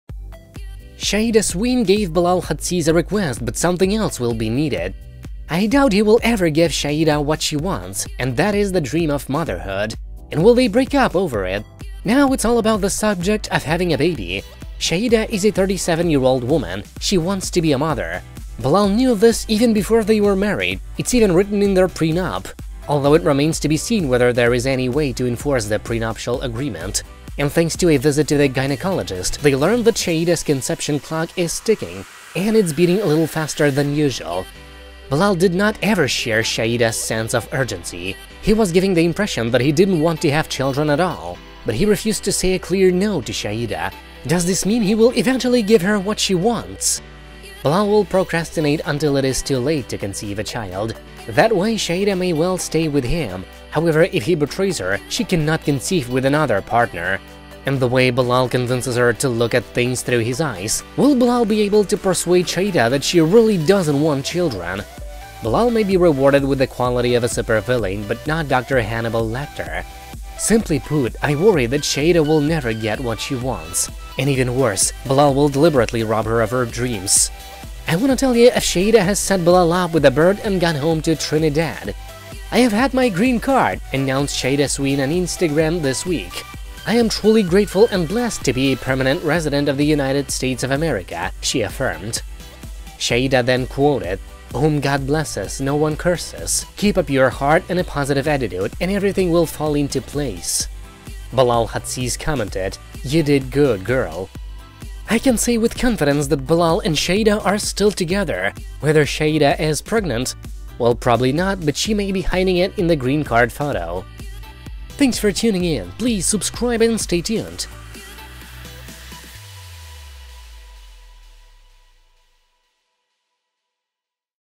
Are Bilal Hazziez and Shaeeda Sween Still Together? - 90 Day Fiance
Shaida Sweene gave Bilal Hazzius a request, but something else will be needed.

I doubt he will ever give Shaida what she wants. And that is the dream of motherhood. And will they break up over it?

Now it's all about the subject of having a baby.
Shaida is a 37-year-old woman. She wants to be a mother.
Bilal knew this even before they were married. It's even written in their prenup... although it remains to be seen whether there is any way to enforce the prenuptial agreement.

And thanks to a visit to the gynecologist, they learned that Shaida's conception clock is ticking. And it's beating a little faster than usual.
Bilal did not and did not share Shaida's sense of urgency.